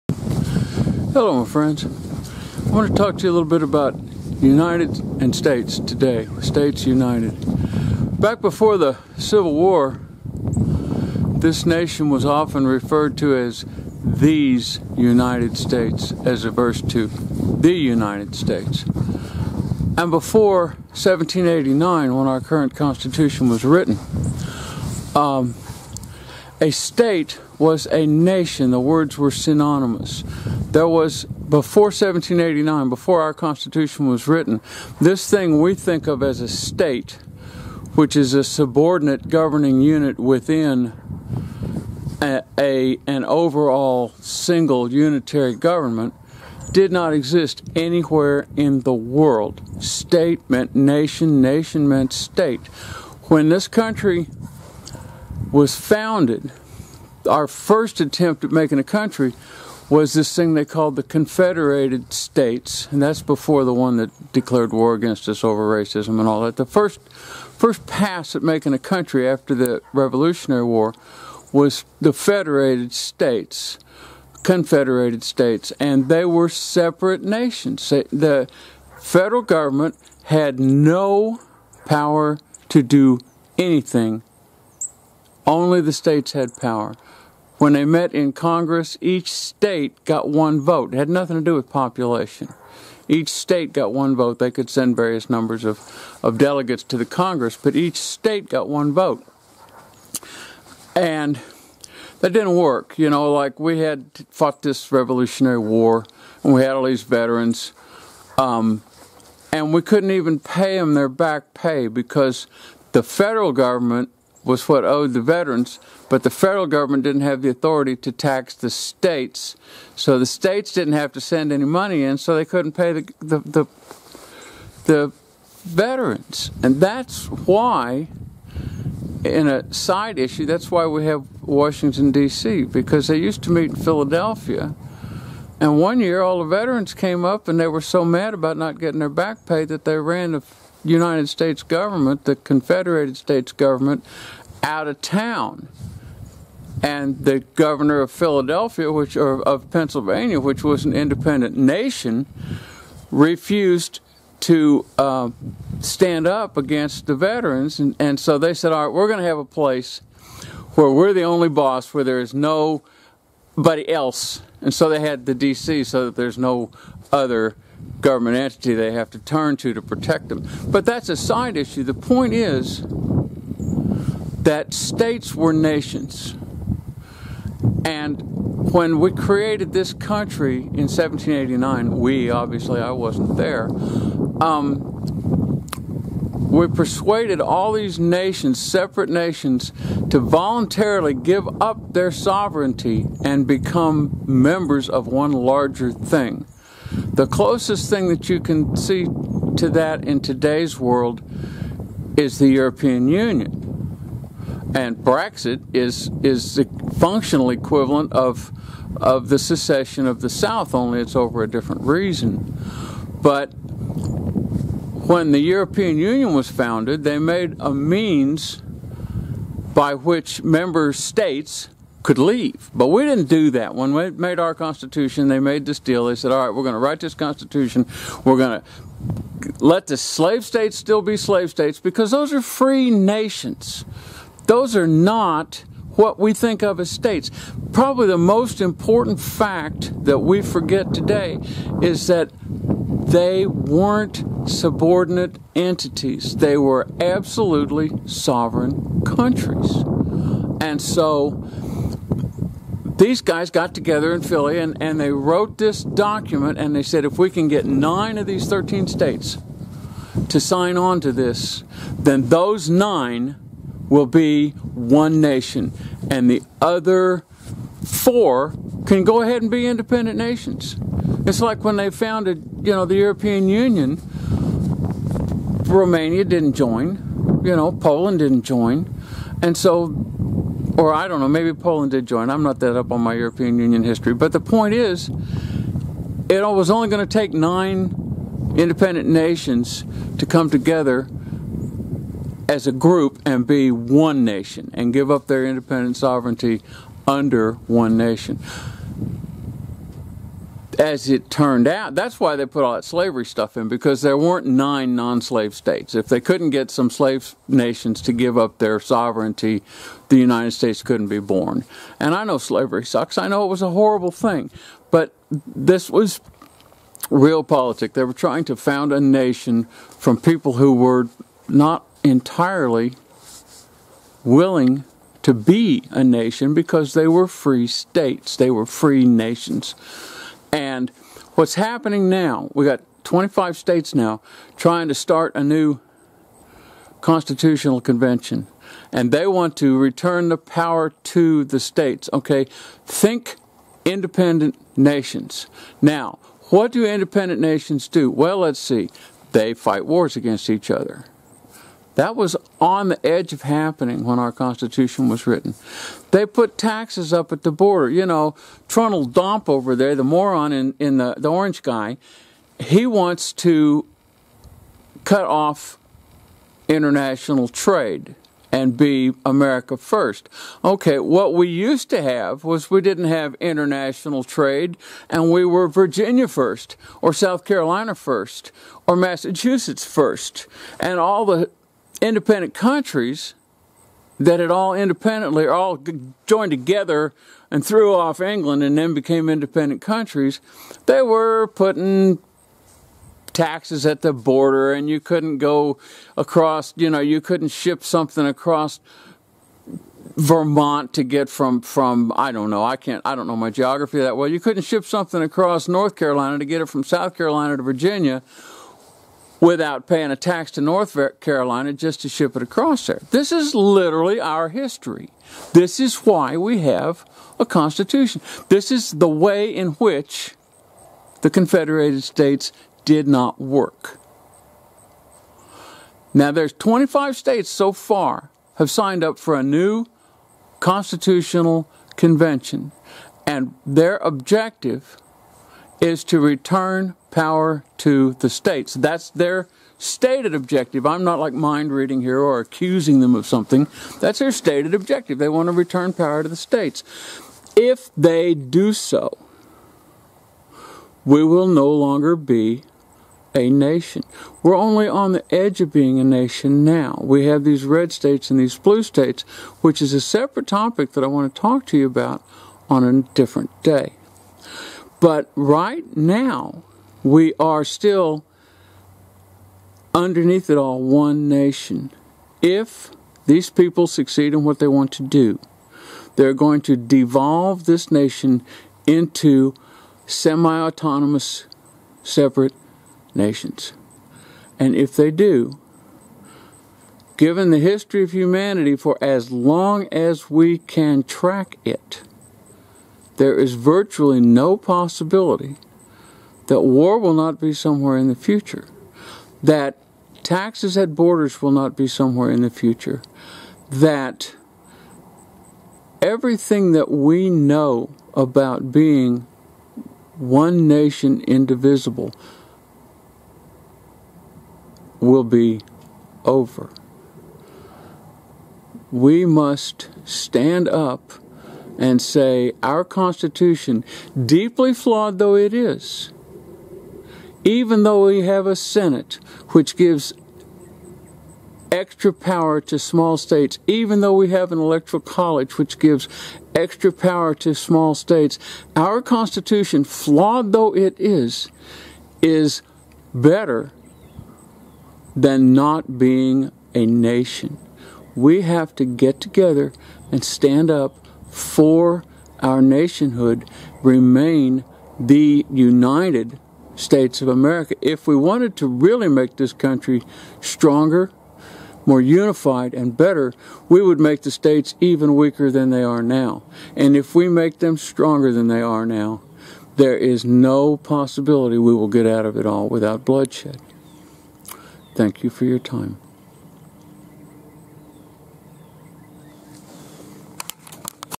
States, Nations, and the Constitution
Shall we be united, or shall we be independent states? I advocate for Union.